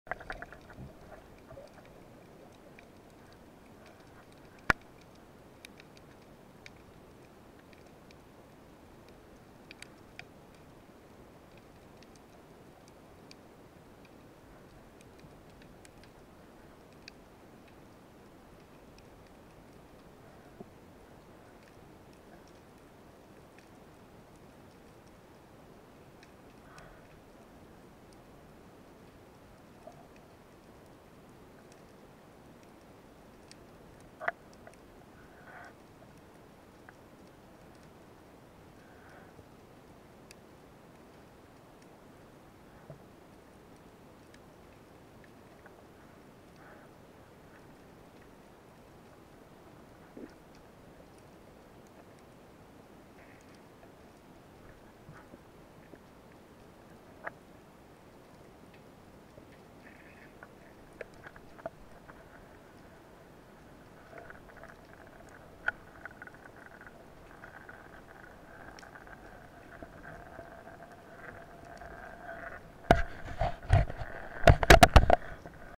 Spearfishing Grouper with Aegean Sea Quest
Spearfishing with @AegeanSeaQuest Groupers, Amberjacks, Dentex and many other kinds of fish on a Private Cruise or Activity Private Trip, to Paros, Antiparos and all around islands